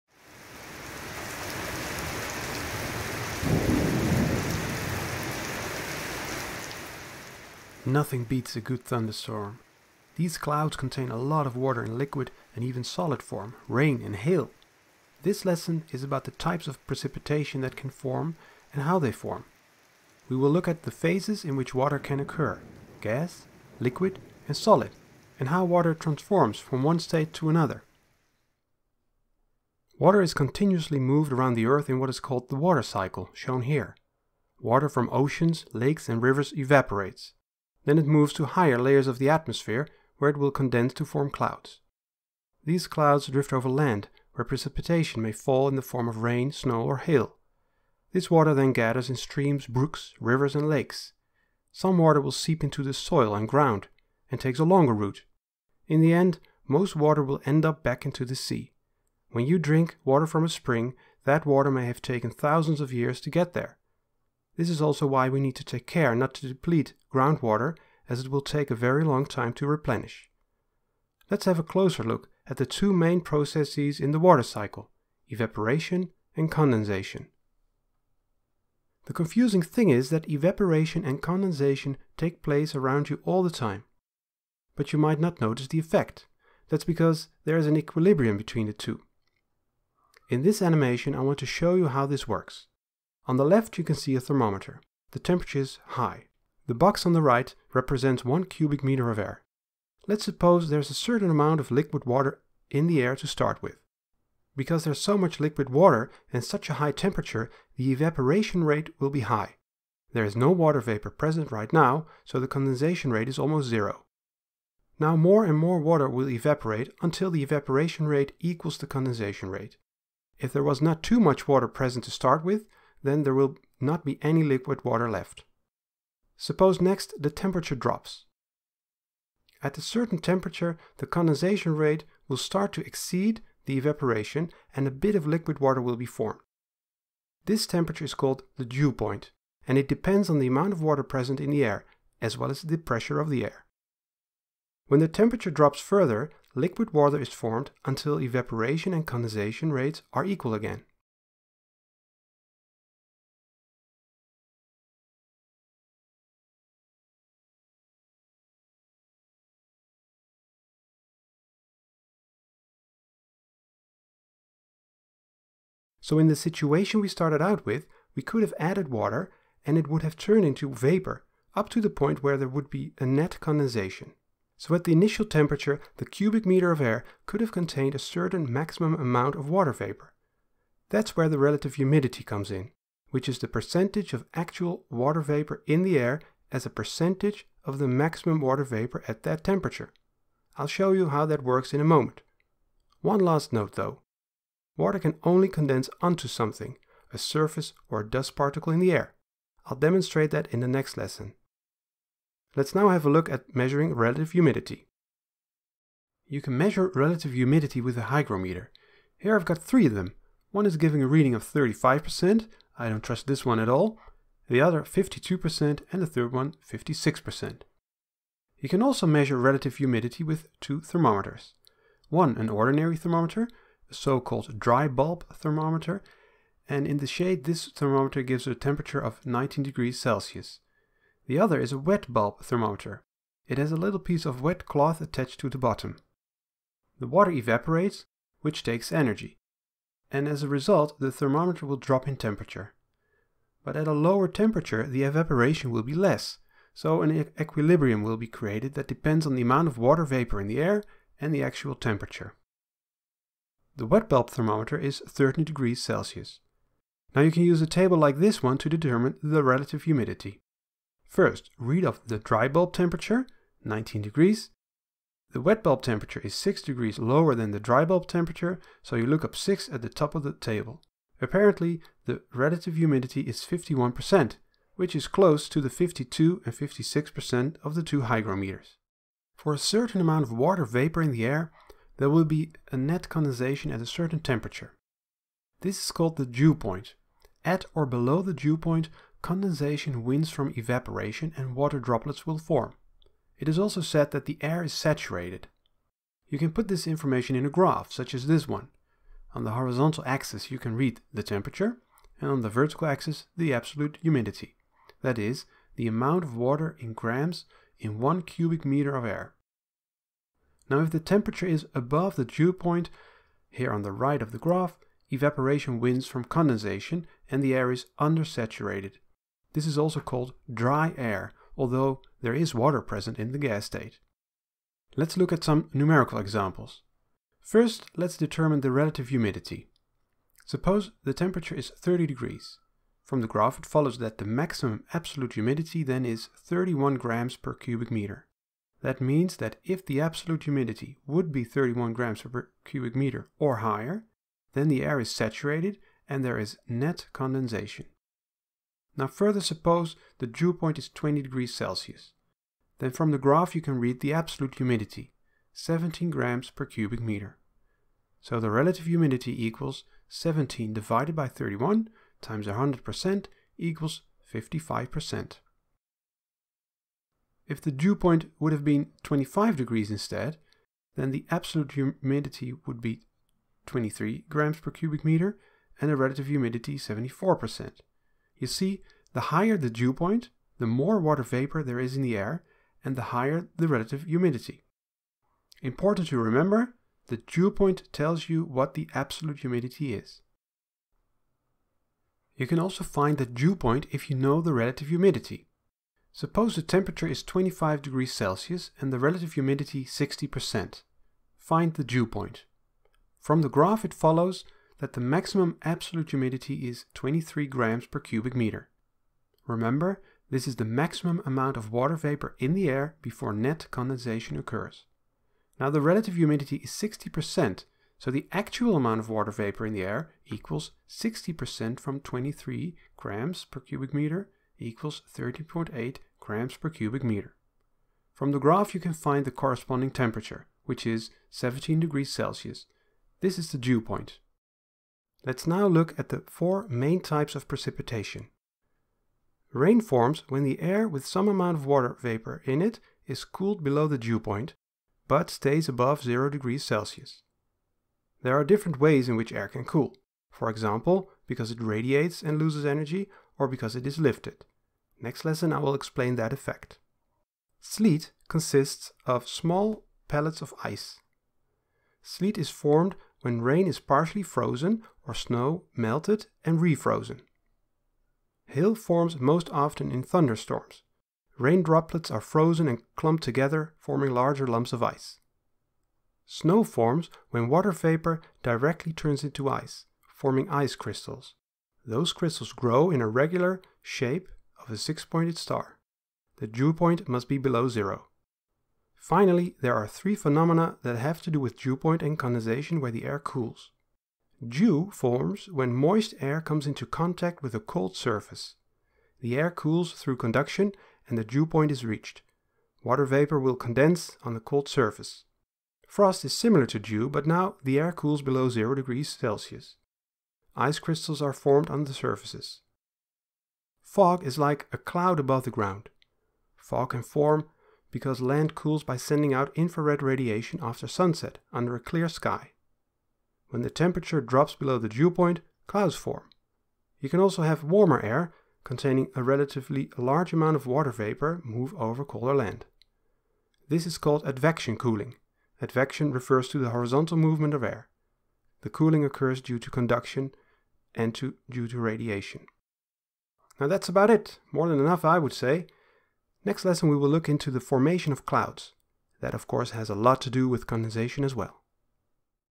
Lesson 4: Evaporation and Condensation
In this lesson I explain the main forms of precipitation though the mechanism of evaporation and condensation. Also I explain the meaning of the dew point and how you can determine the dew point.

Image credits:
- Thunderstorm: Mathias Krumbholz (Wikimedia)
- Water cycle: NASA/JPL
- Animation: own work
- Rain: W. Carter (Wikimedia)
- Sleet: Runningonbrains (Wikimedia)
- Hail: Raysonho @ Open Grid Scheduler / Grid Engine
- Snow: Oleg Magni from Pexels.com
- Snowflake: Glkvs2 (Wikimedia)
- Dew: Rajatkantib (Wikimedia)